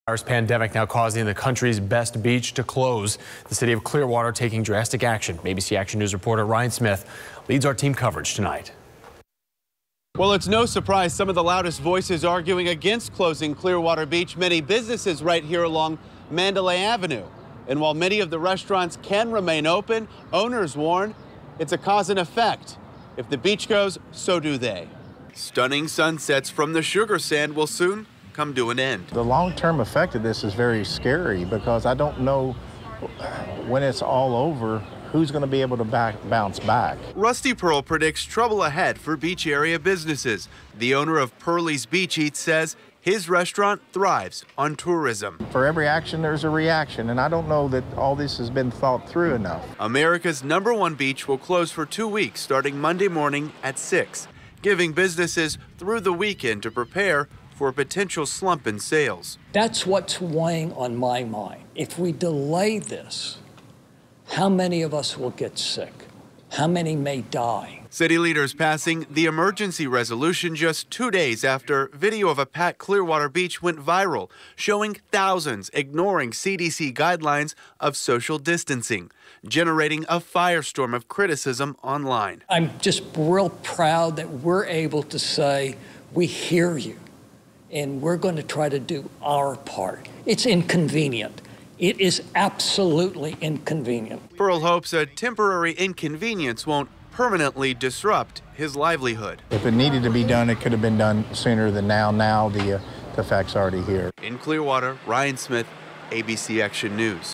City votes to shut down Clearwater Beach for two weeks amid coronavirus concerns
The announcement comes days after videos showing spring breakers flocking to the beach became viral.